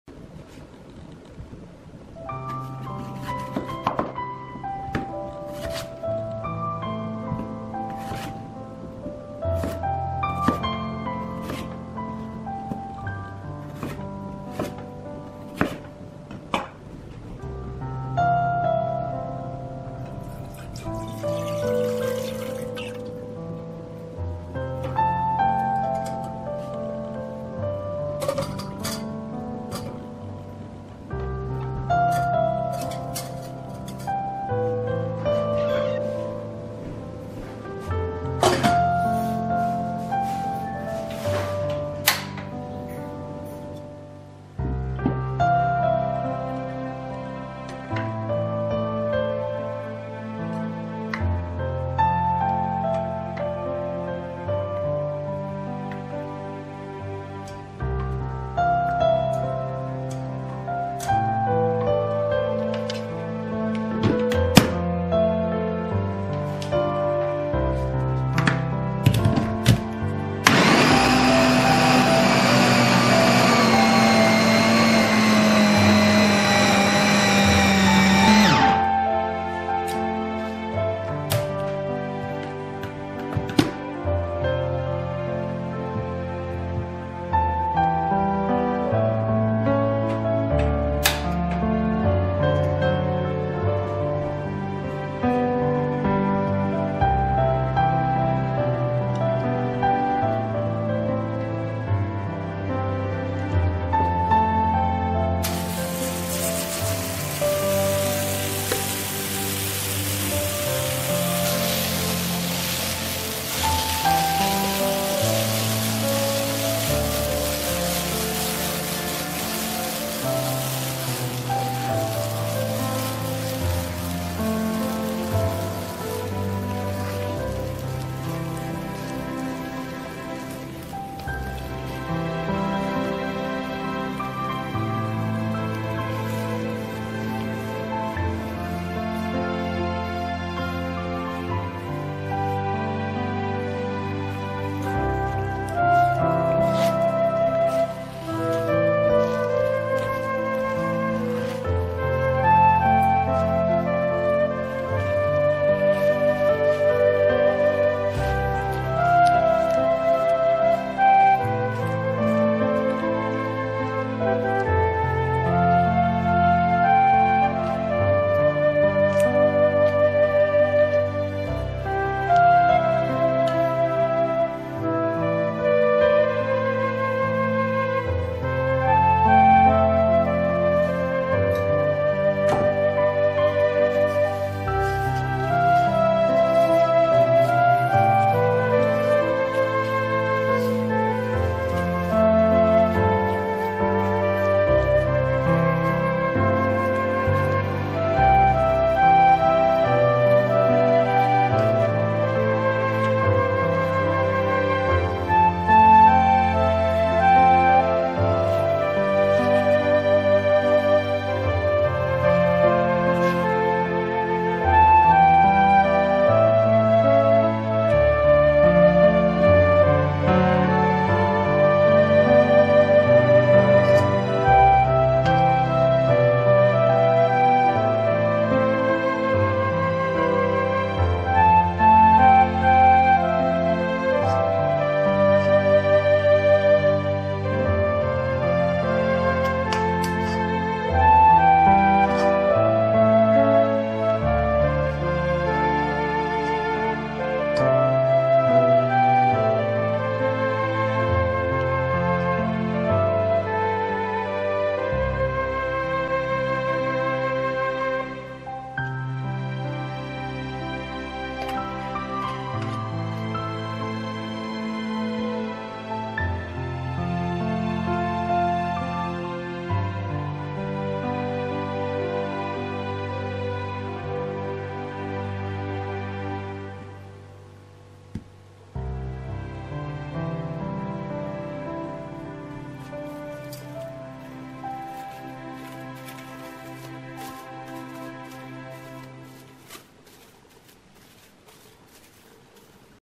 香芋馅 How to make Taro Desert Filling
简单健康，自家制，再也不用担心吃香精和添加剂